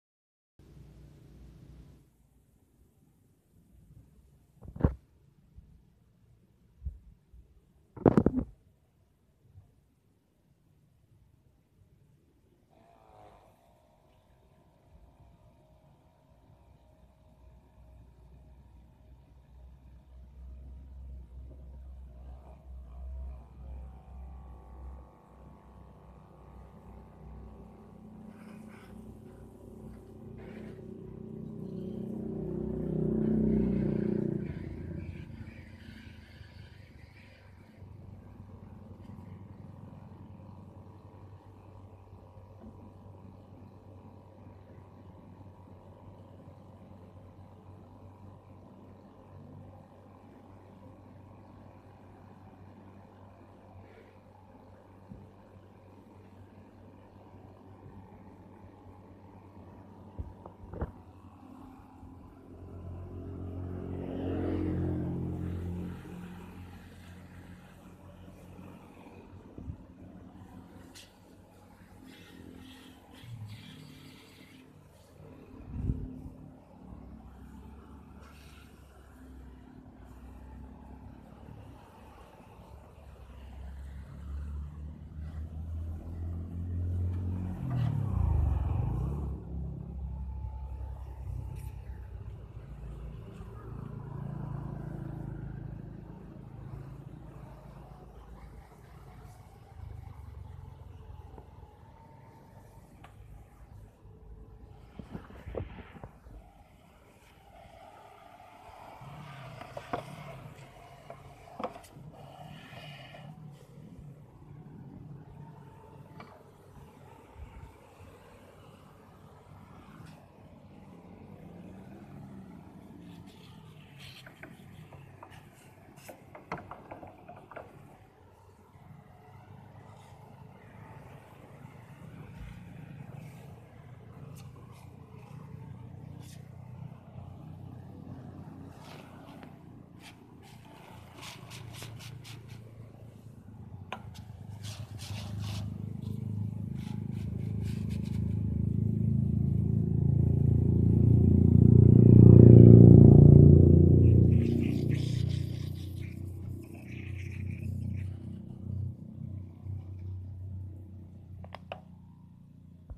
cuci motor listrik
cuci motor listrik dengan pompa dc 12 volt.Cucinya bagian yang tidak ada kabelnya